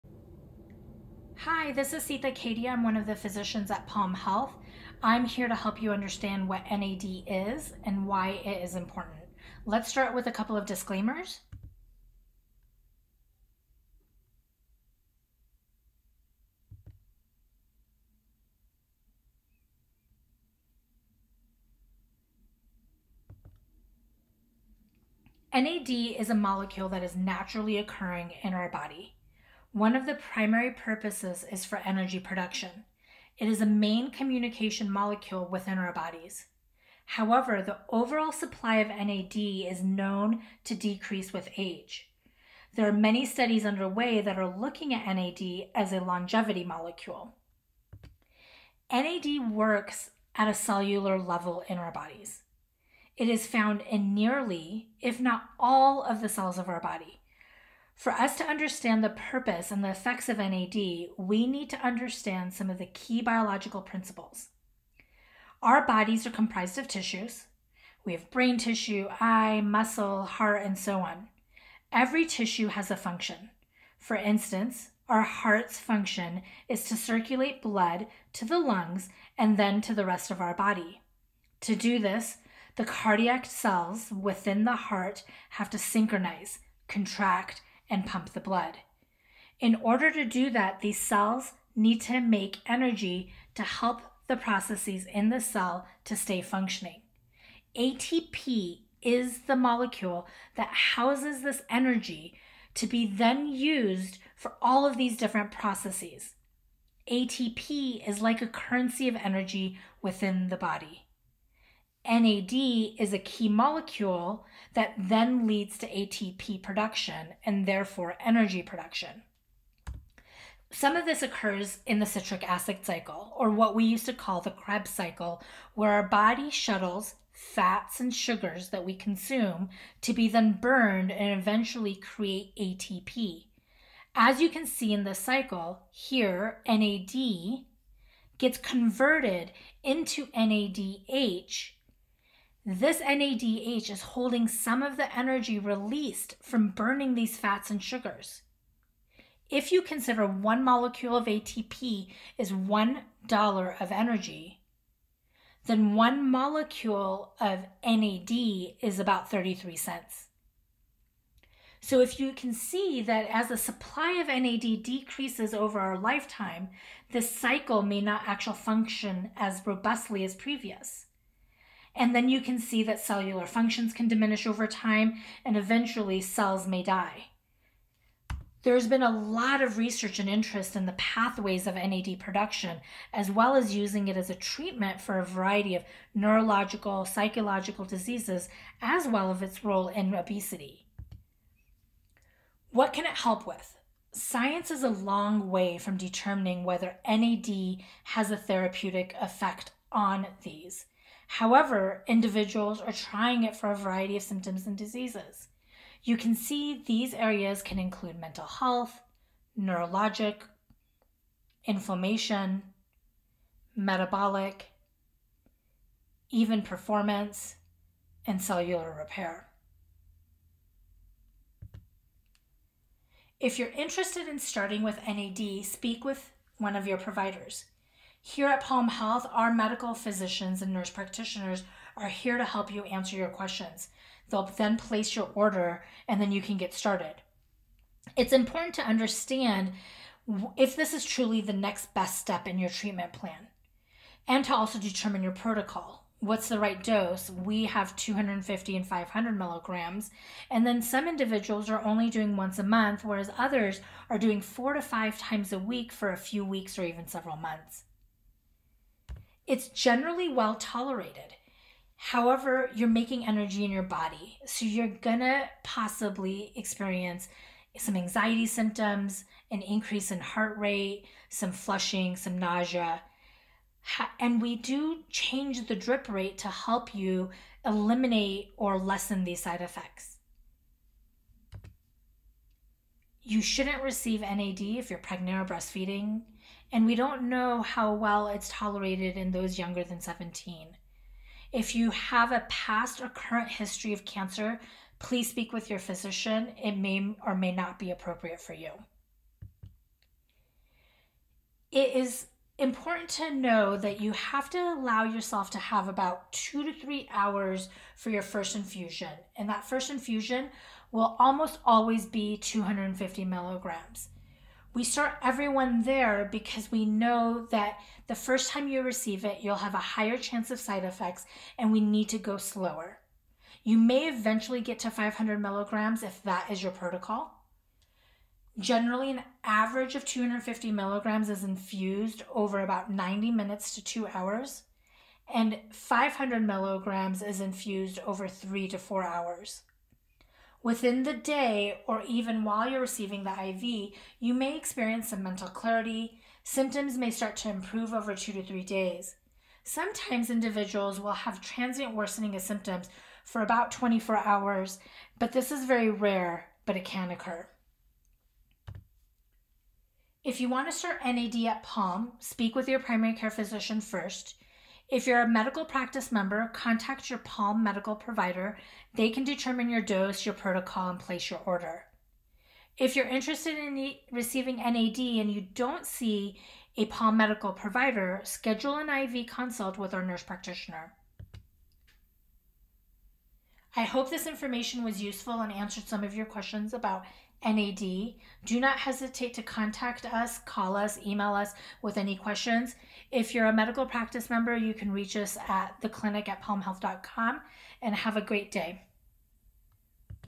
The Science Behind NAD+ IV Therapy
Low energy? Trouble focusing? Sluggish metabolism? NAD+ IV Therapy is a cutting-edge infusion that revs up the engine in your cells, giving you more energy, helping you think more clearly, and boosting your metabolism, among other benefits.

Listen as Dr. Sita Kedia explains the science behind NAD+ IV Therapy. As you age, your NAD+ (nicotinamide adenine dinucleotide) reserve in your body decreases. Age-associated diseases such as Alzheimer’s, diabetes, and heart disease are known to be associated with depletions of NAD+ levels within the body. This naturally occurring biological compound is a critical component to the basic function of cells and can be administered intravenously.